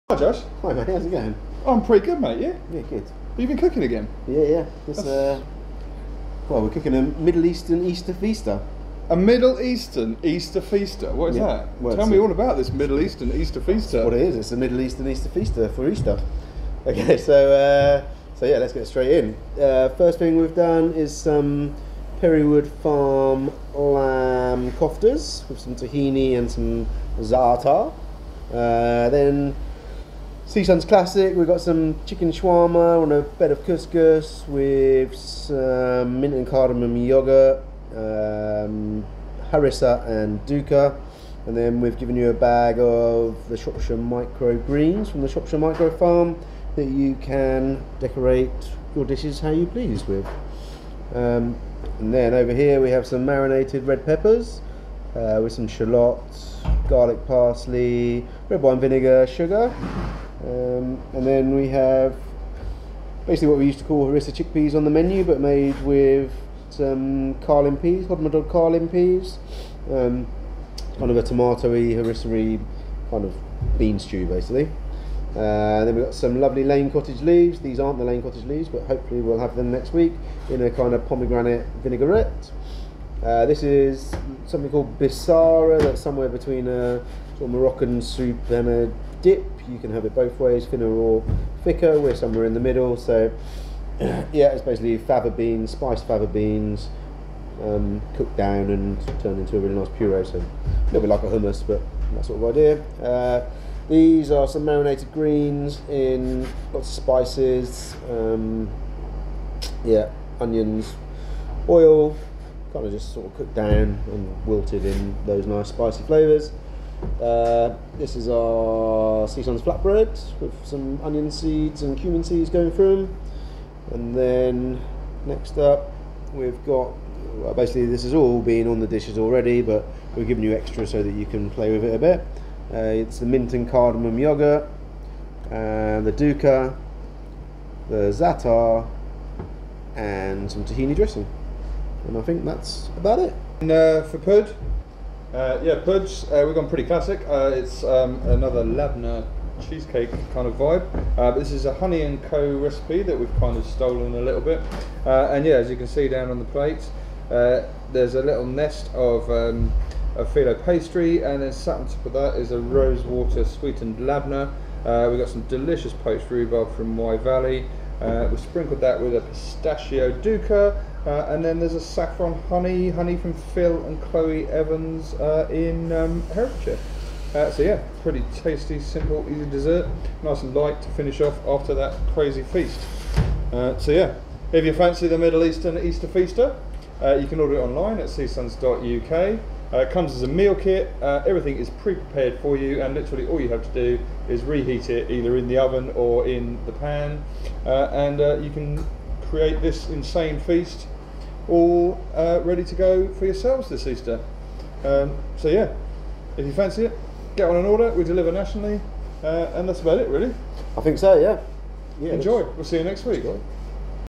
CSONS AT HOME - The Middle Eastern Easter Feaster Menu Overview
A quick run down of what's on the menu for our Easter menu. Like what you see? You can order our meal kits for collection in Ludlow or Shrewsbury and NATIONWIDE delivery. at home for details and to order online!